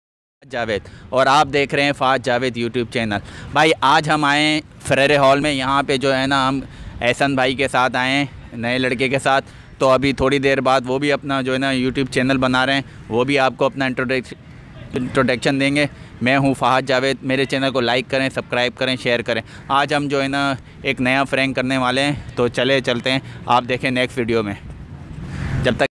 farrer Hall Meet And Greet New Vlogger
This video Is about for Introduction for new one vlogger YouTuber